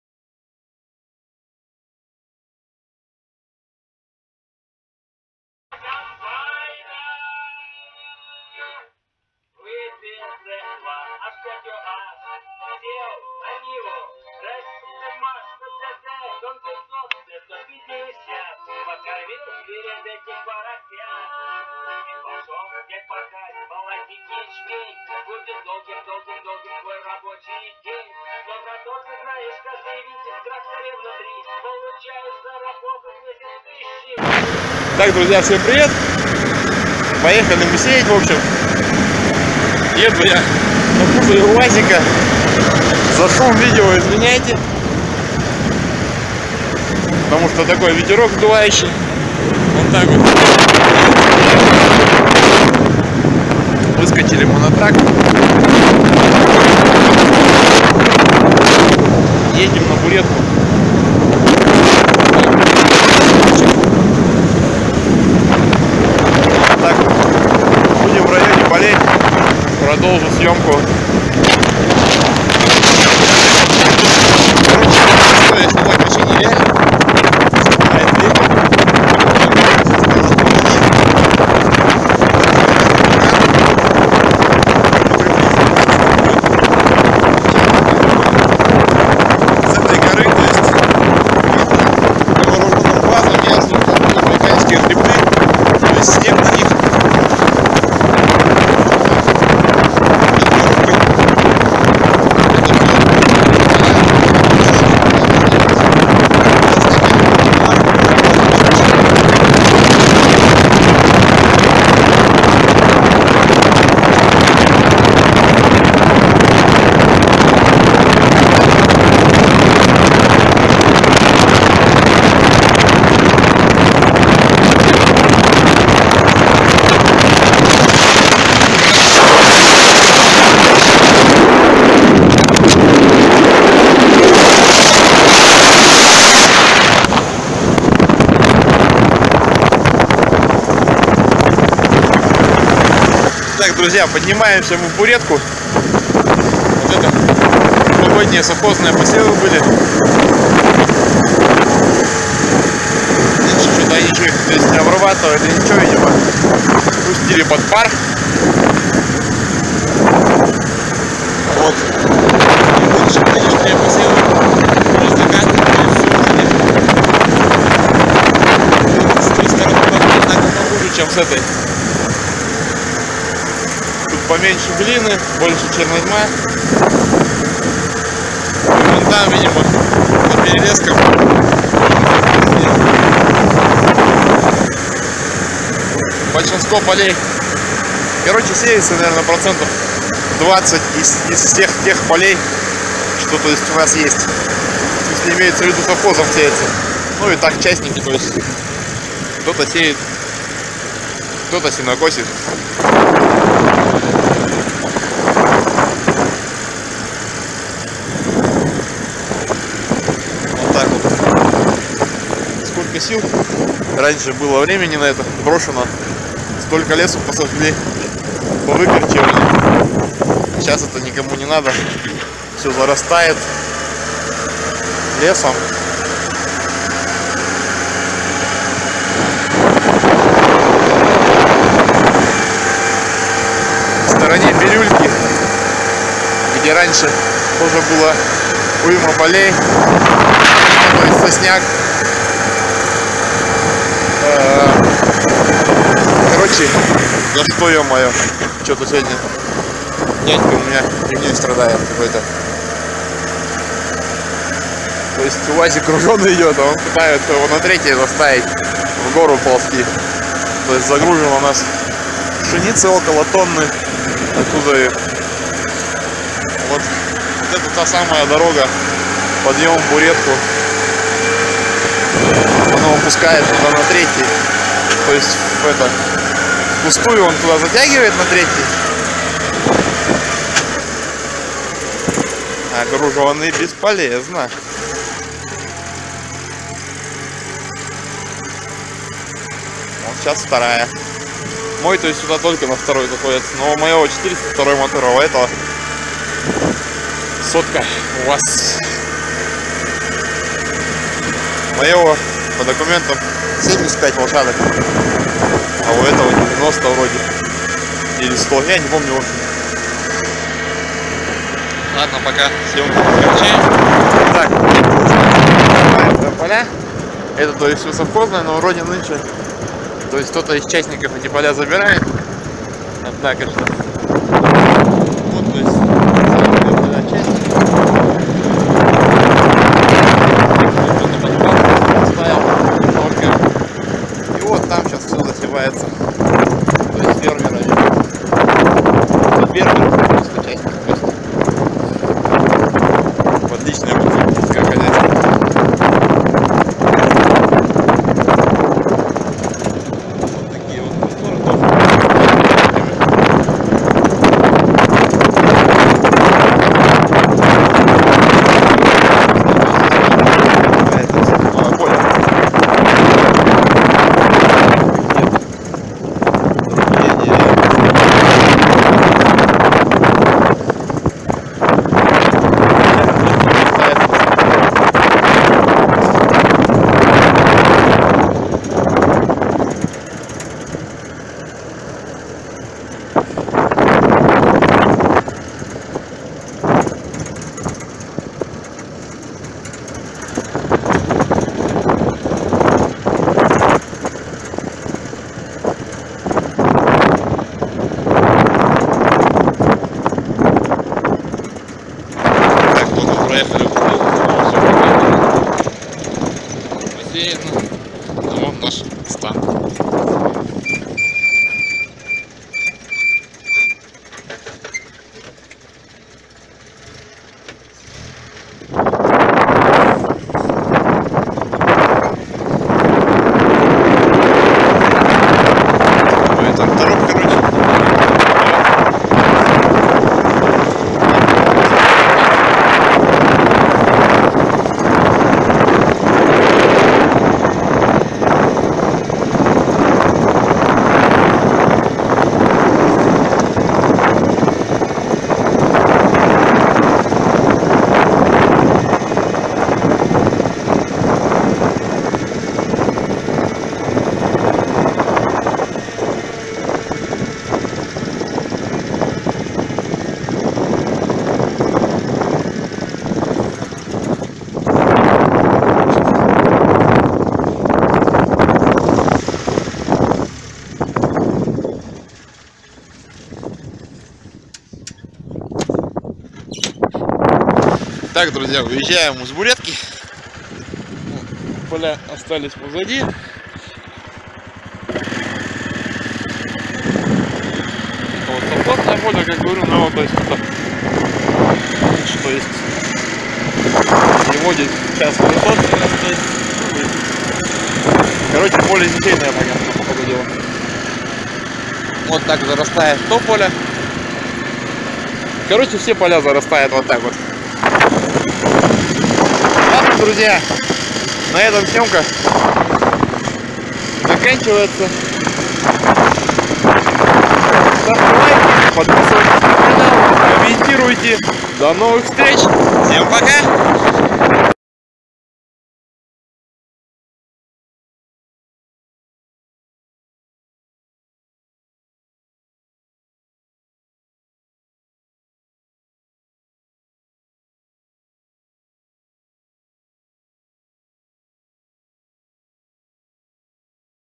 Квадрацикл СТЕЛС АТВ 500, Едим на посевную, поговорим о С/Х.
Поехали на посевную, осталось немного,ну  за качество как всегда извиняйте , Я еду по квадрик, надо посмотреть коней